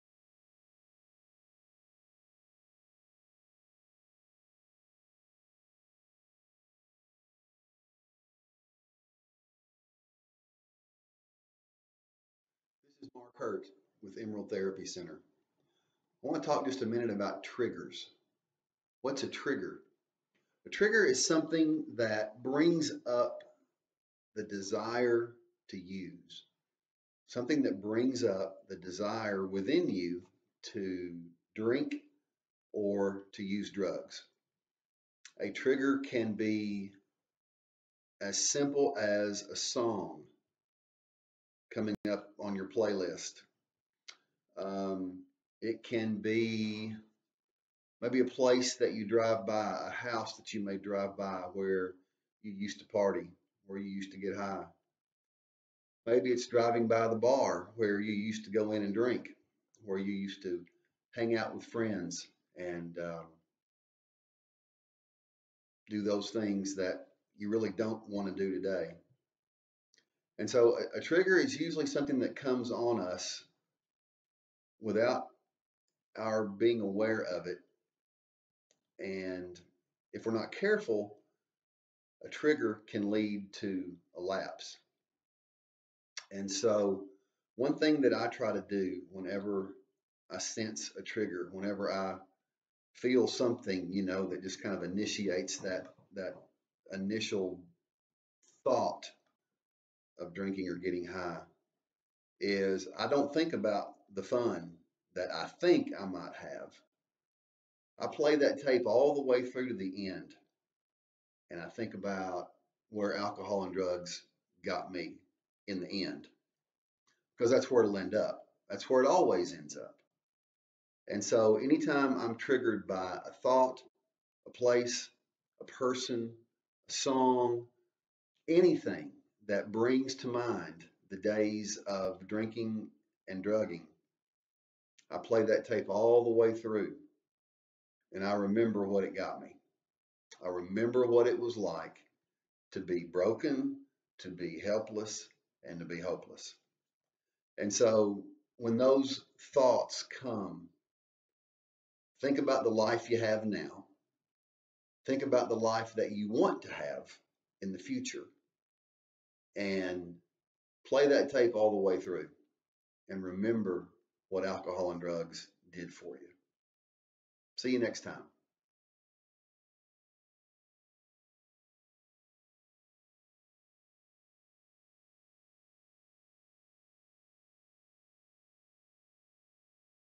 Emerald vlog Series: Triggers
Mark Hurt from Emerald Therapy Center shares some advice for those who may be struggling with substance abuse issues or in recovery during COVID-19.